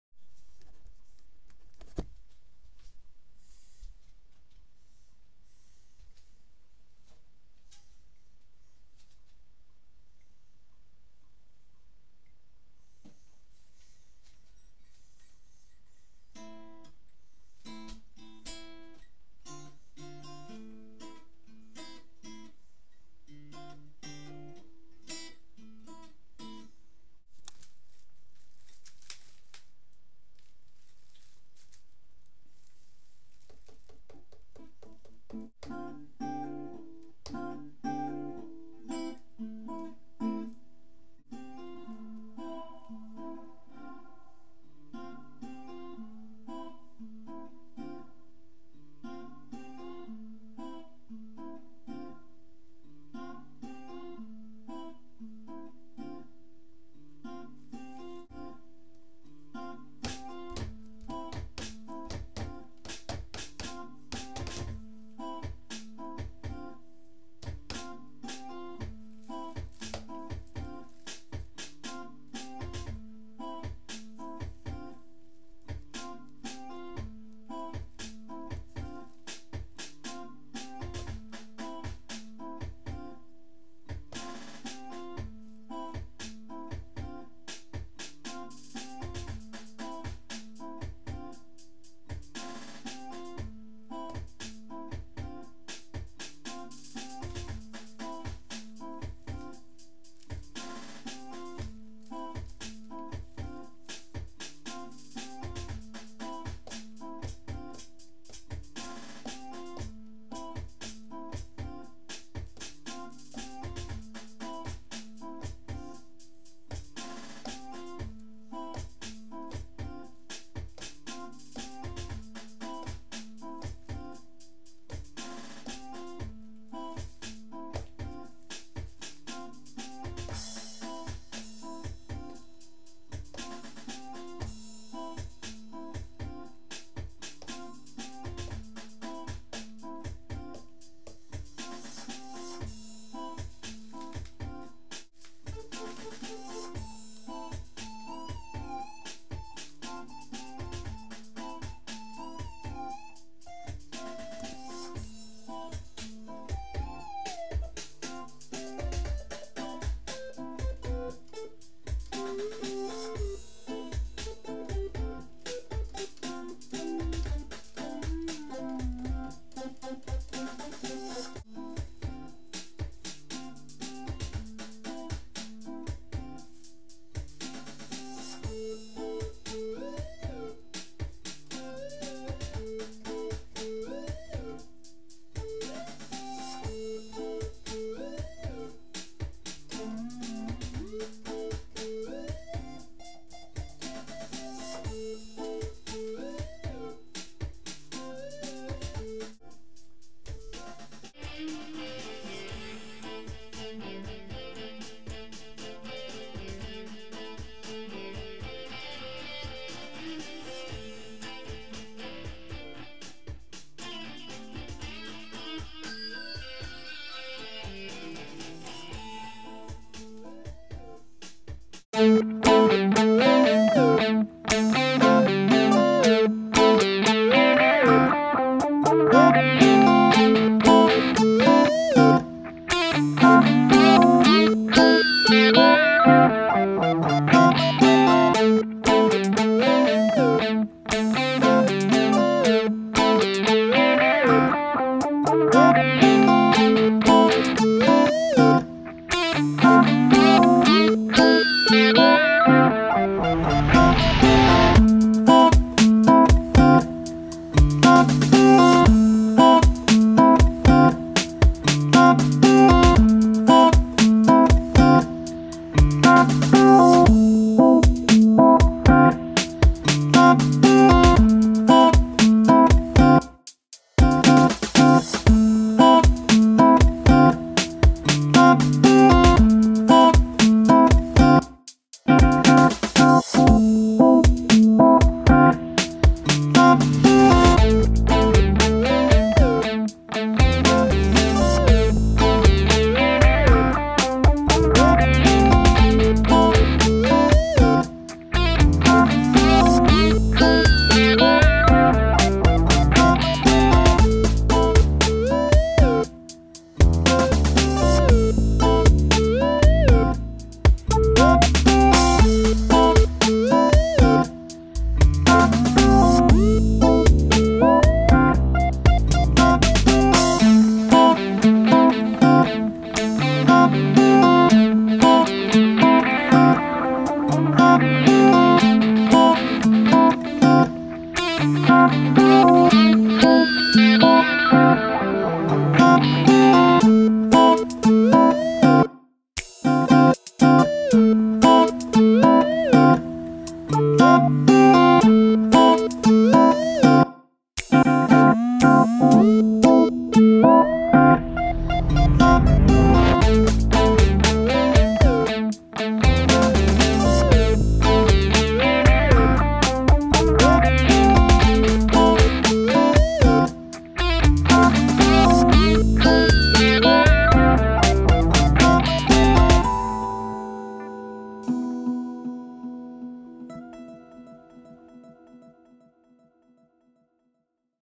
Fresh Kicks
**CAUTION** : you may want to adjust your volume at 3:42!! It gets loud!! The track is mixed a lot louder than the footage of the beat being recorded.

This is my first attempt at a beat making video!  I show most of the parts being written, and the beat plays during the last 2 1/2 minutes.
I did do some behind the scenes editing and mixing, as well as adding a bassline.  I'm working on getting a screenshot recorder to show my work on the computer for the next one.
_T-walk_